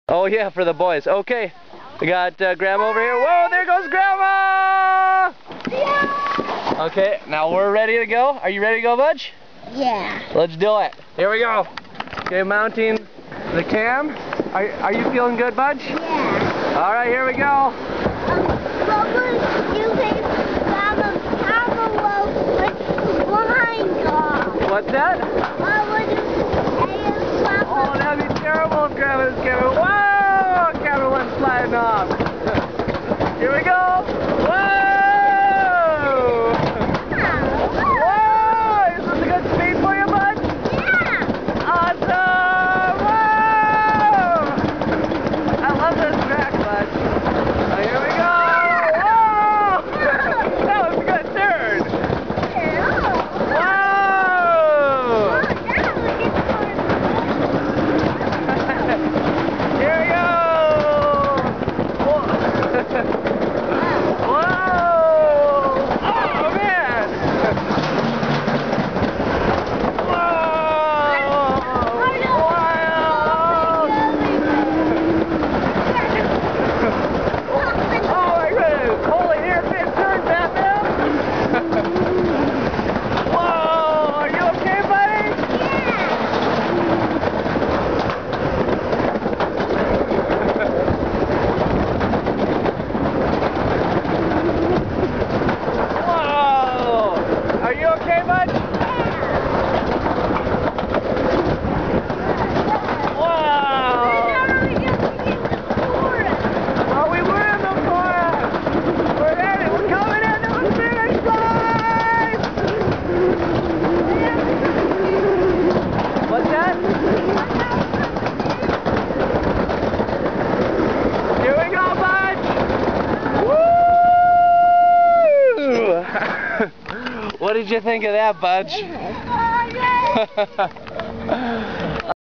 Sommerrodelbahn (Bobsledding in the the summer) in Austria
Andreas and I had a great time going nearly full speed down a 1.3 Km stainless steel bobsled run in the Salzkammergut area of Austria.